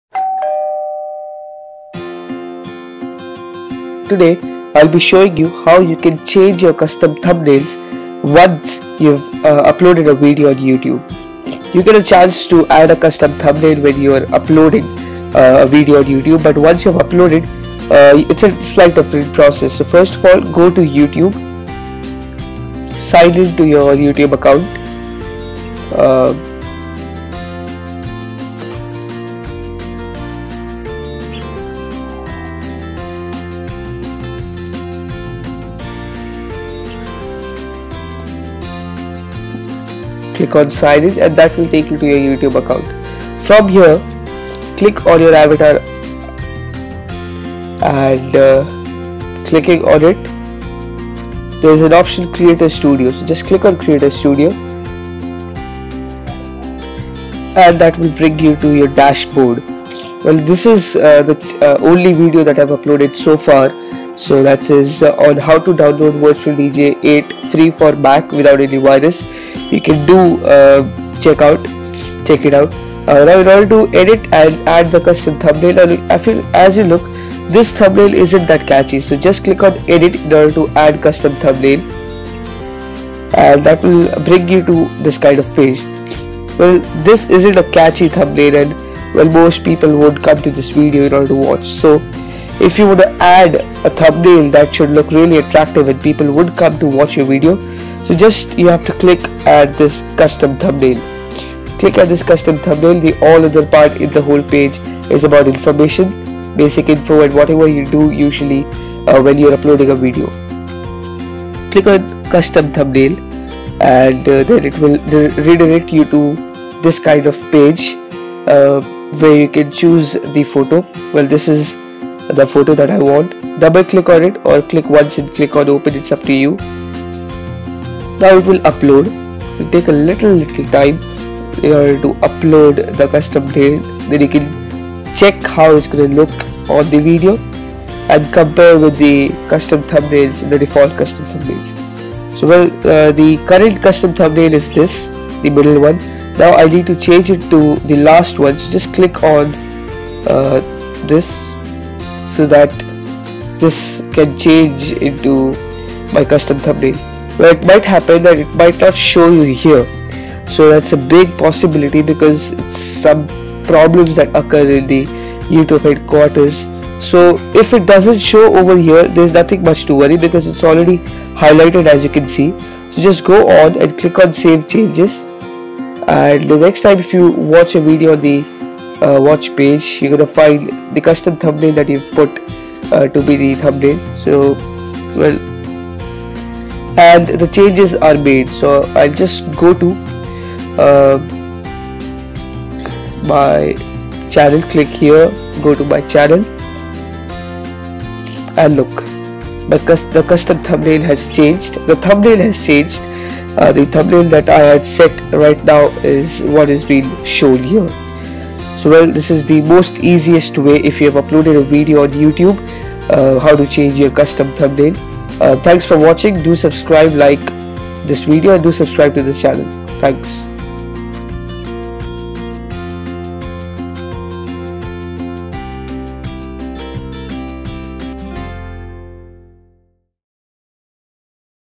How To Change Thumbnails once you have uploaded a video on Youtube
How To Change Thumbnails once you have uploaded a video on Youtube | The How-To Expert
You wanna change your youtube thumbnail after you have uploaded the video to youtube, then do watch this video..